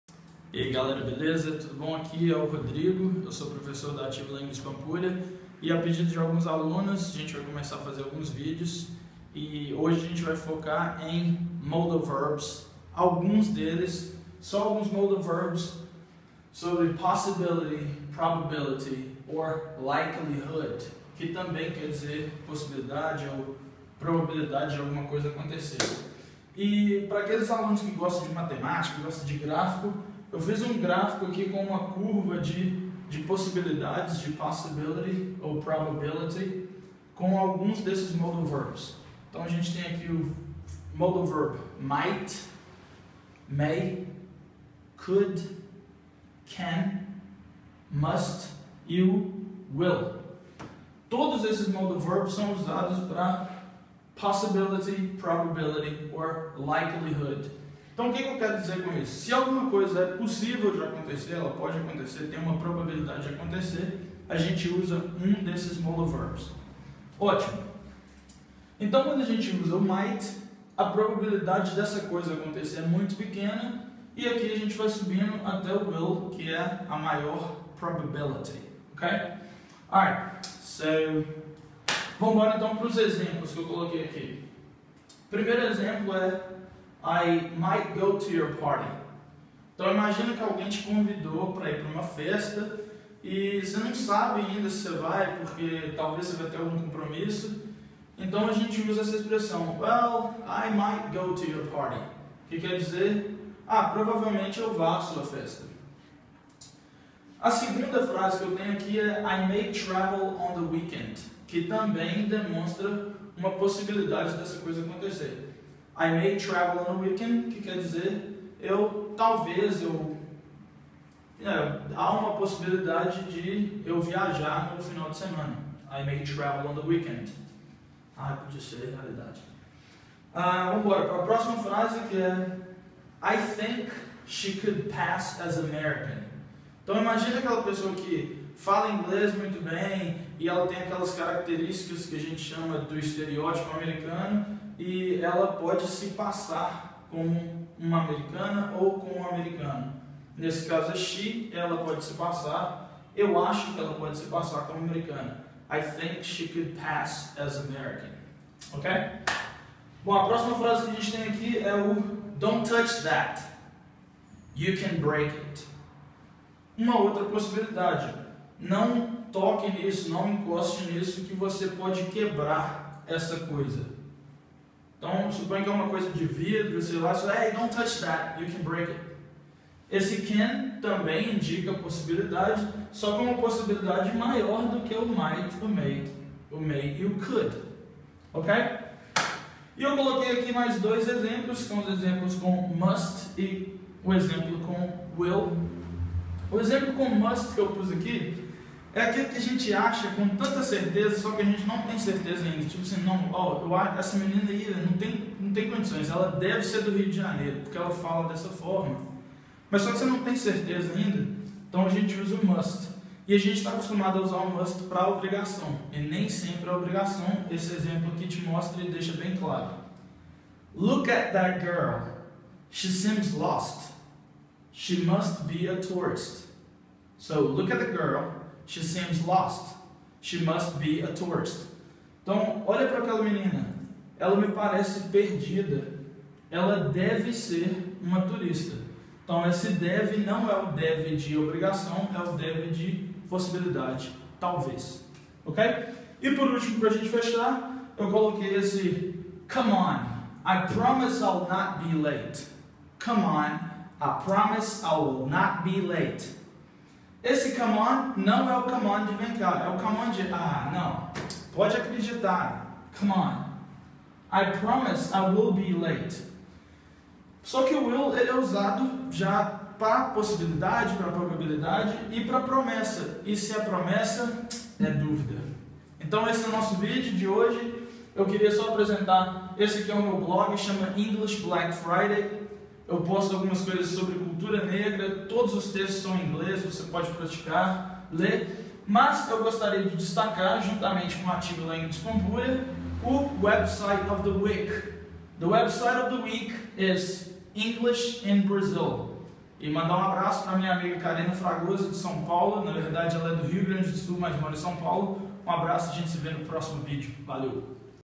Modal Verbs (Possibilidades)
Hello! Aprenda mais um pouco sobre o uso de alguns Modal Verbs para possibilidade ou probabilidade.

Comentem para que eu possa melhorar cada vez mais e compartilhar mais coisas legais sobre a lingua inglesa.

Um abraco,
Rodrigo P. Honorato

Website of the week:
Carina Fragozo's Blog: